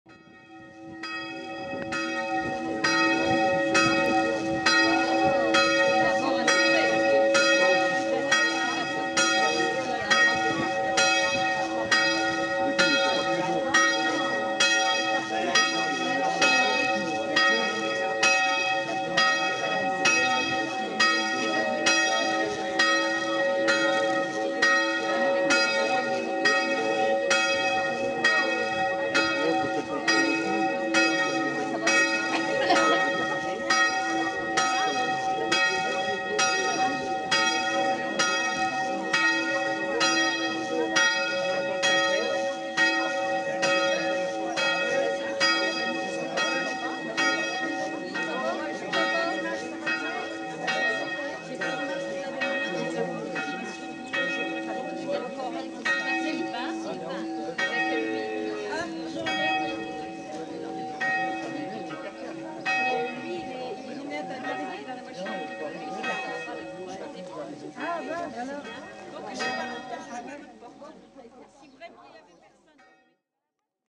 La cloche de Boncourt, hameau de Fléchin dans le Pas-de-Calais
La cloche de l'église du hameau de Boncourt a un son très particulier. De l'argent aurait été fondu avec le bronze. Cette cloche date de l'année 1400.